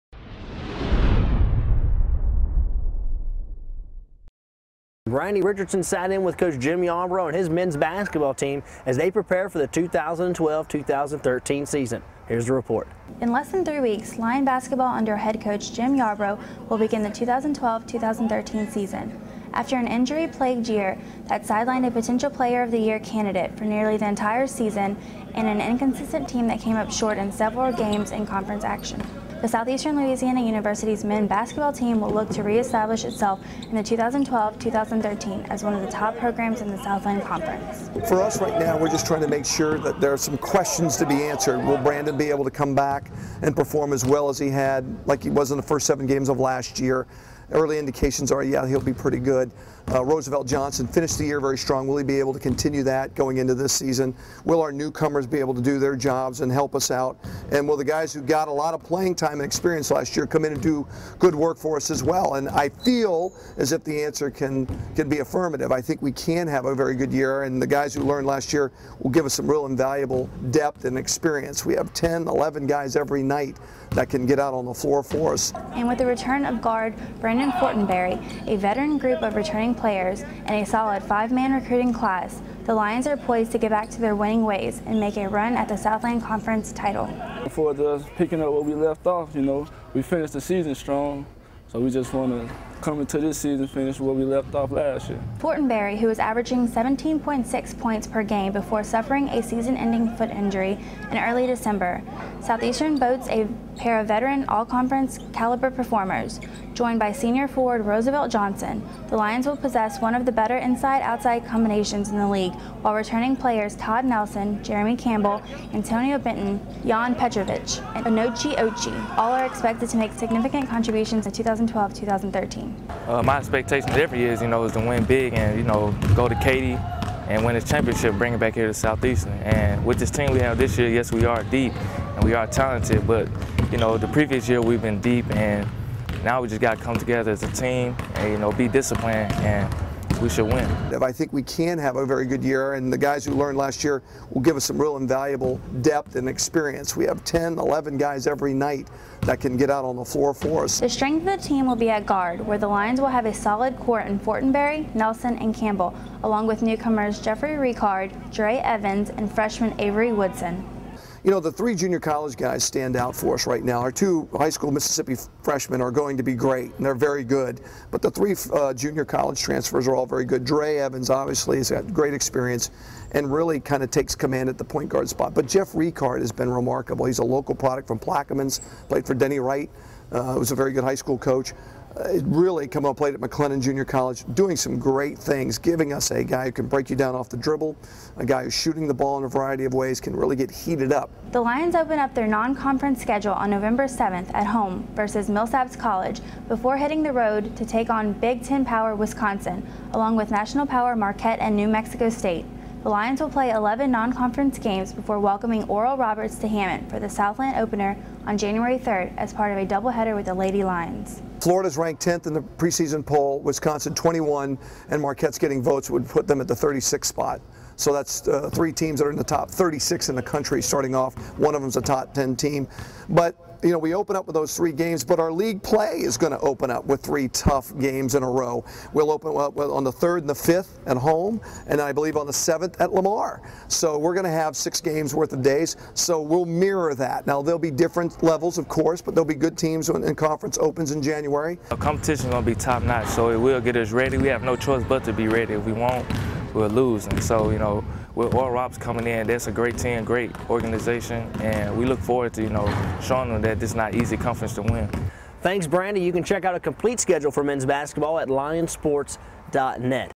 MBB: Southeastern Louisiana Preview
A non-conference schedule that features three teams that finished in the Associated Press Top 25, six games against teams that appeared in the NCAA Championships and a revamped Southland Conference schedule that includes newcomer Oral Roberts highlight the 2012-13 Southeastern Louisiana University men's basketball schedule.